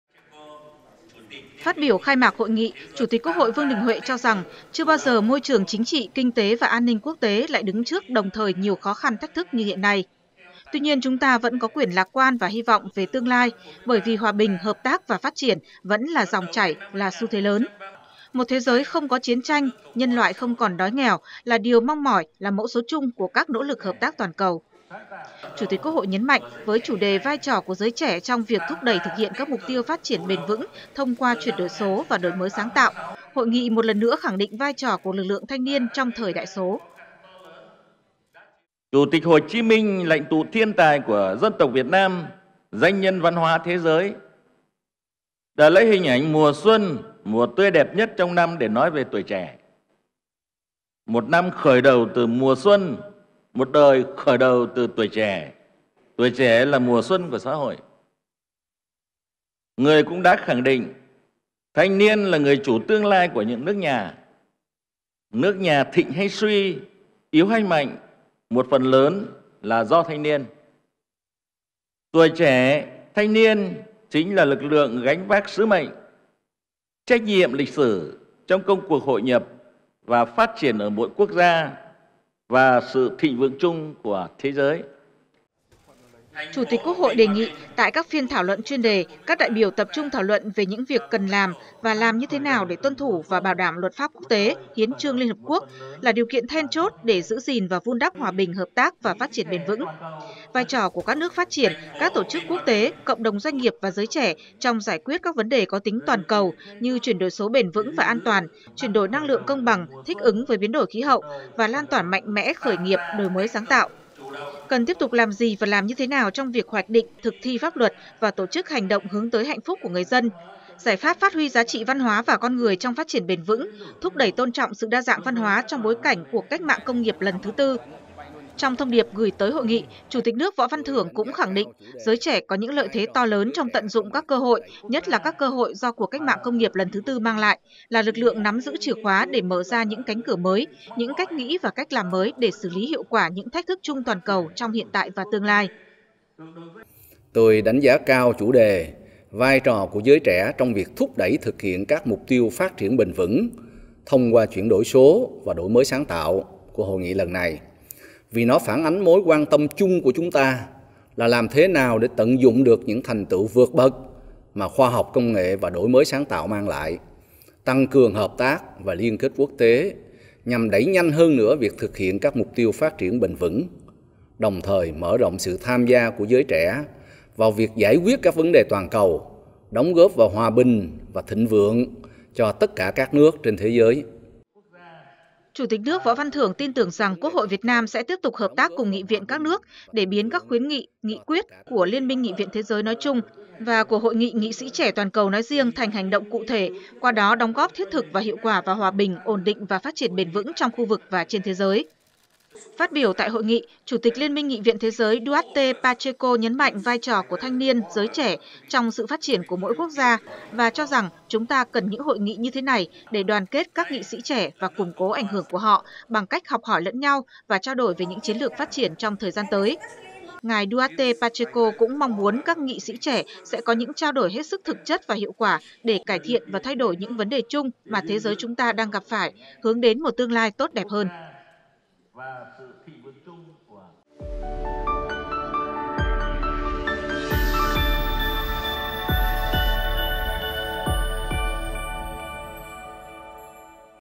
Khai mạc Hội nghị Nghị sĩ trẻ toàn cầu lần thứ 9 | VOV
Tổng Bí thư Nguyễn Phú Trọng, Chủ tịch nước Võ Văn Thưởng và Thủ tướng Chính phủ Phạm Minh Chính gửi lẵng hoa chúc mừng Hội nghị.

Tham dự phiên khai mạc Hội nghị, về phía đại biểu quốc tế có ngài Duarte Pacheco, Chủ tịch Liên minh Nghị viện Thế giới (IPU); ngài Martin Chungong, Tổng thư ký IPU; ngài Dan Carden, Chủ tịch Diễn đàn Nghị sĩ trẻ IPU, Hạ nghị sĩ Vương quốc Anh; bà Cynthia Lopez Castro, Chủ tịch Diễn đàn Nữ nghị sĩ IPU.